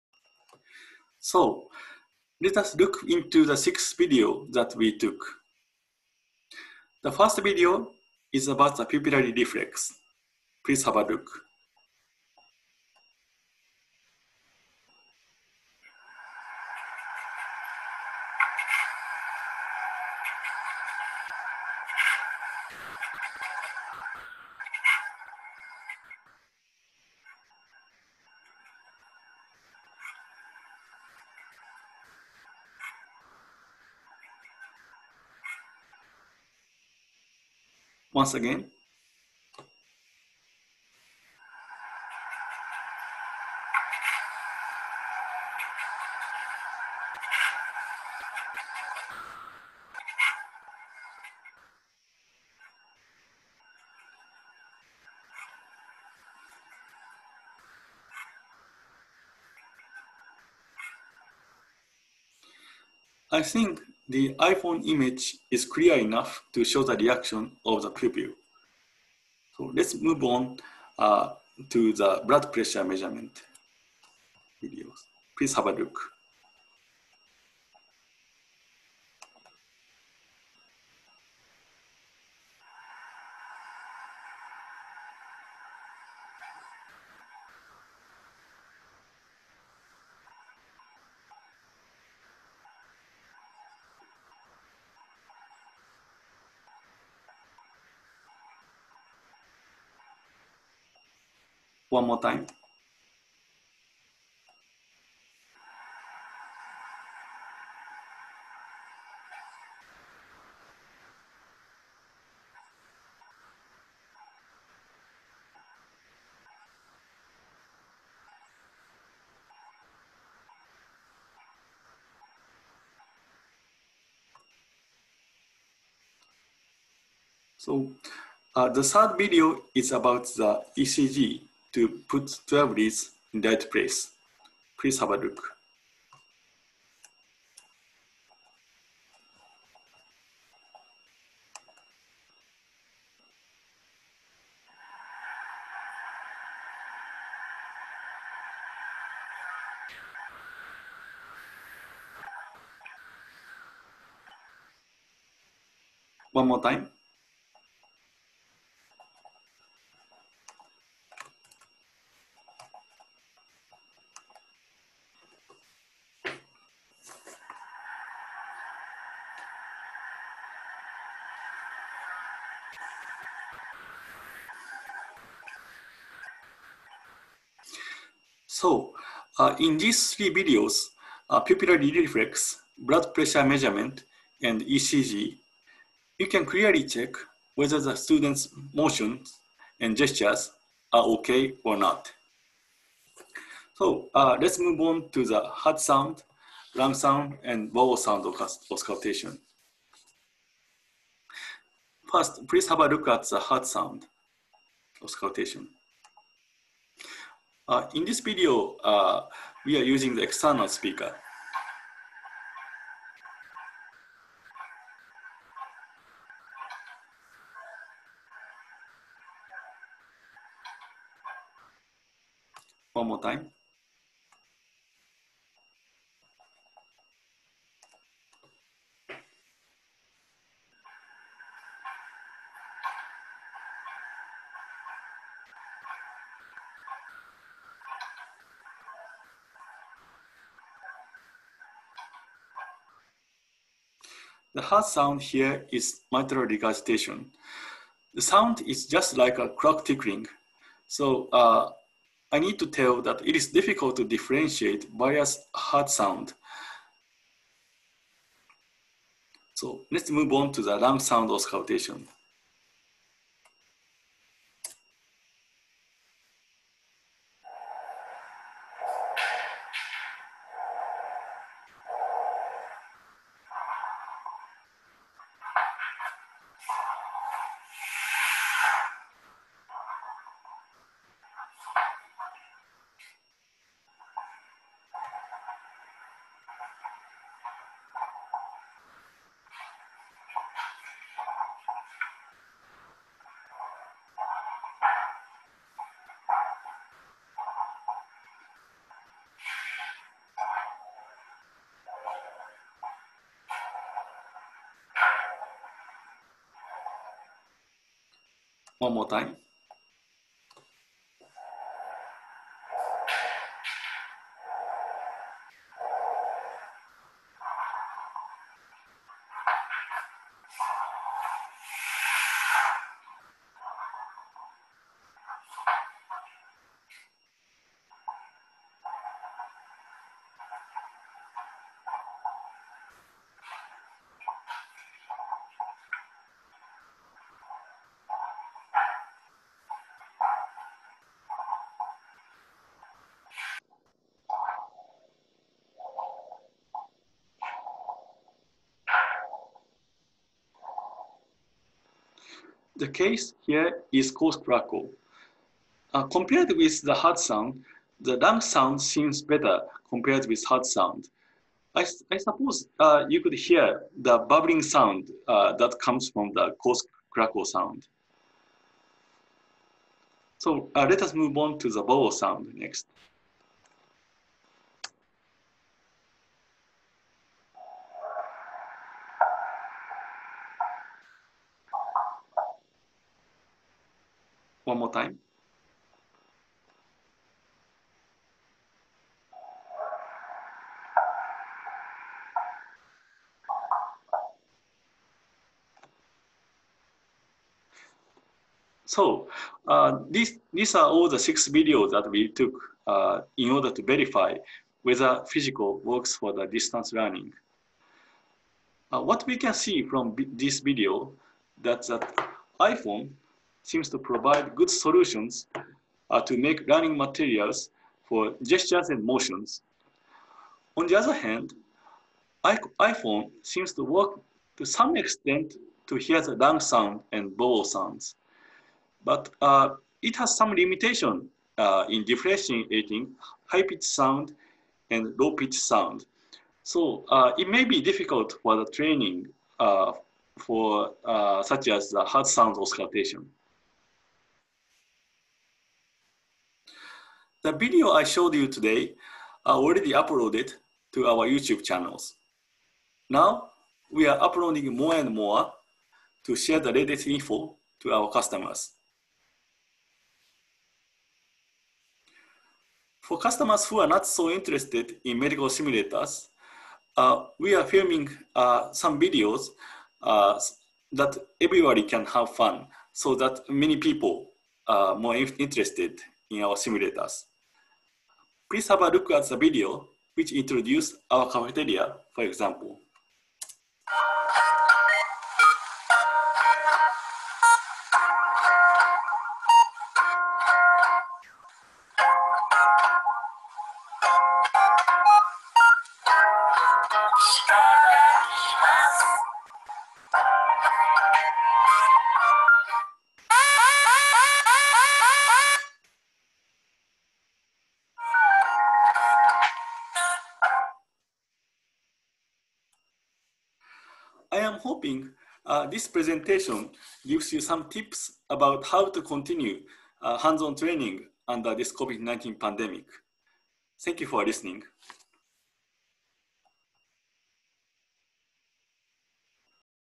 Presentation about how to use "Physiko" for Distance Learning Part 2/2
Presentation for Webinar held in Indonesia
Clinical Skills Laboratory Webinar Series
Improving the Quality of Clinical Skills Lab Activity during the Pandemic Era